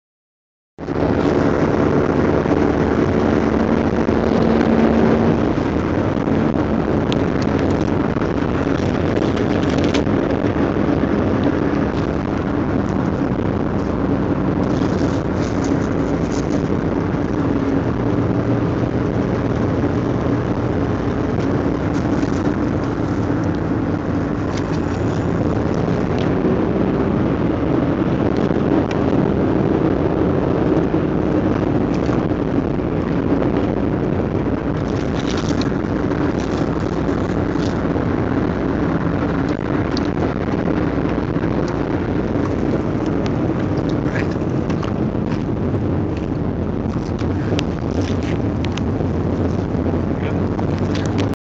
Cab ride down the mountain back to oaxaca
Stressful but fun